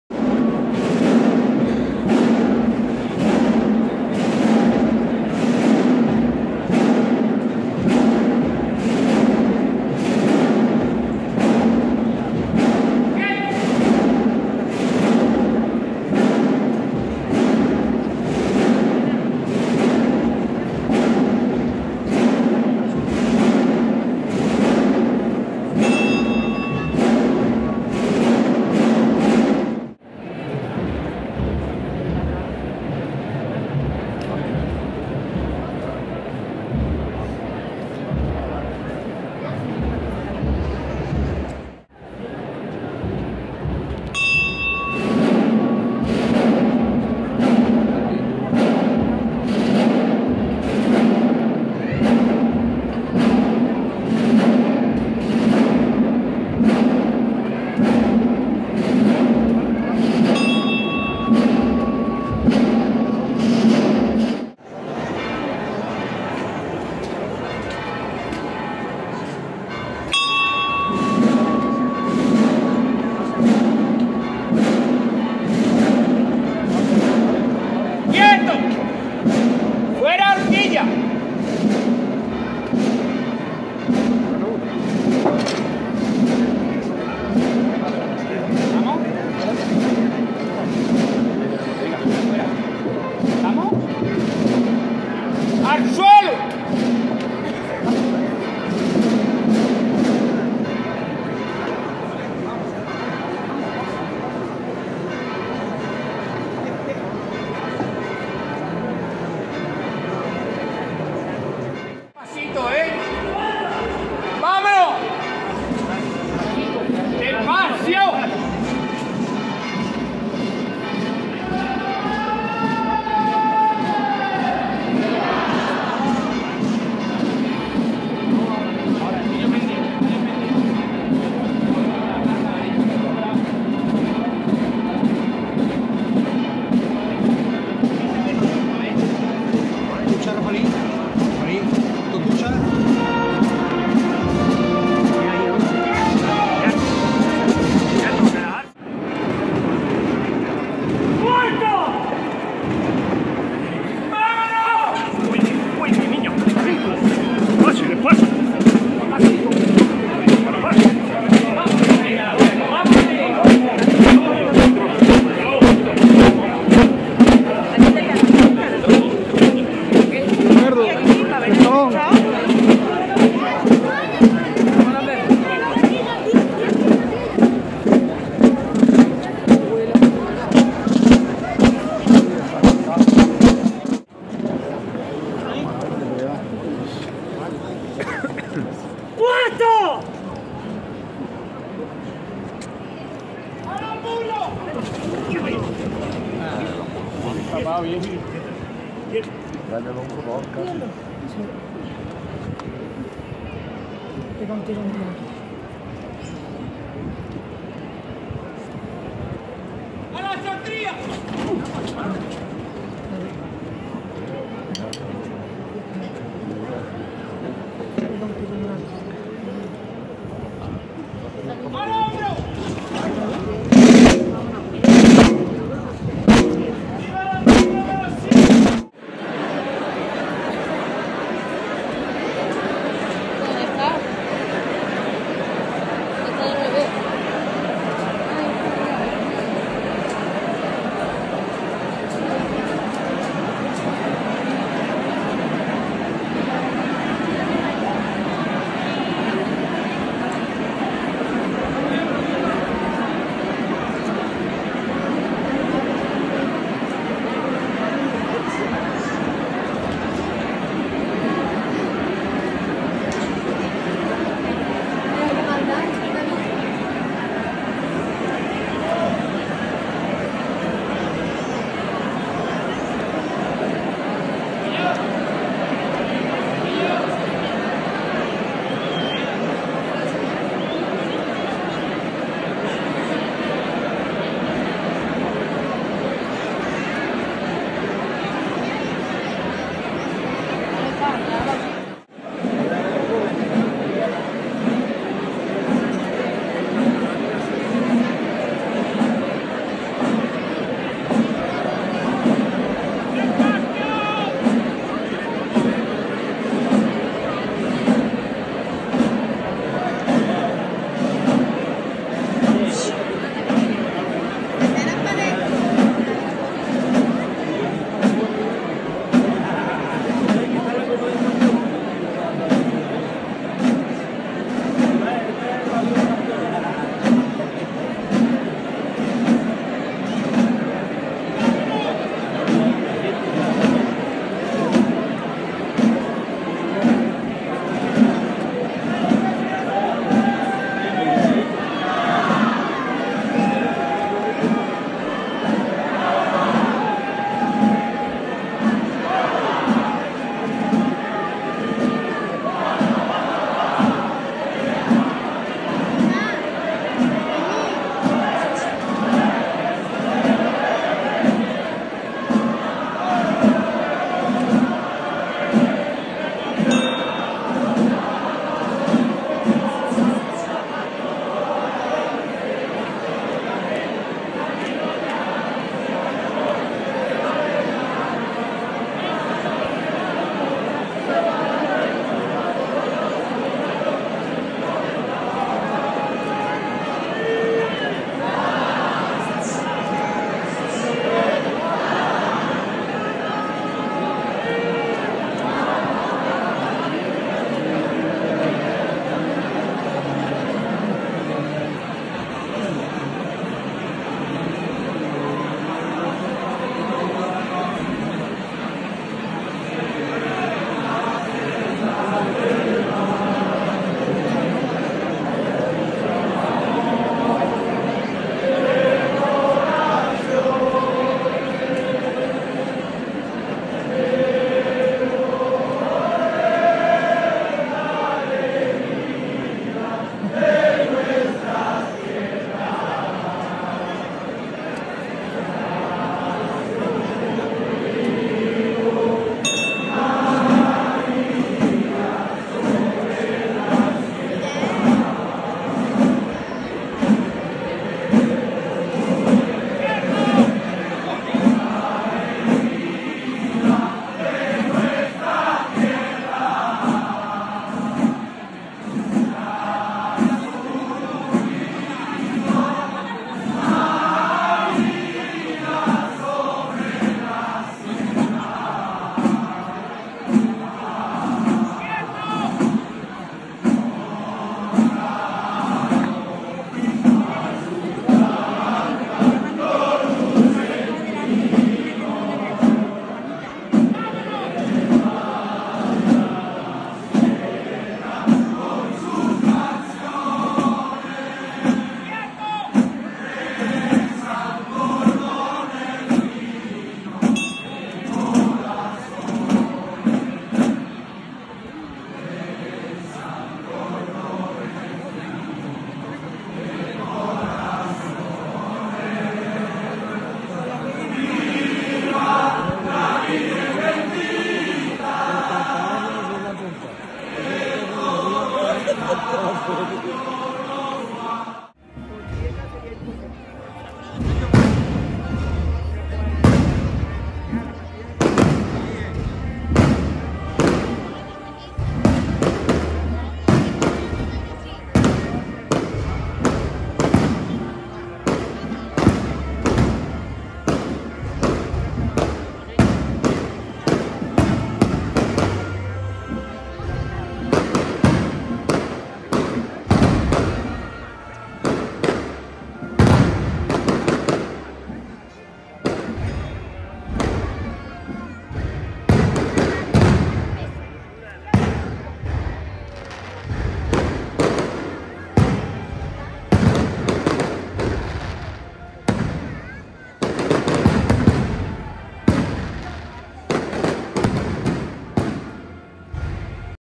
2024 DÍA DE LA VIRGEN DE ARACELI CORONADA. Primer domingo de mayo, día 5. Lucena Córdoba.
PROCESIÓN DE MARÍA SANTÍSIMA DE ARACELI CORONADA, Patrona de Lucena y del Campo andaluz.
5 de mayo de 2024.
Lucena Córdoba.

📹 ElParigolón
📹 Francisco de Paula Romero Ruiz
📹 Antonio Carrera Calvo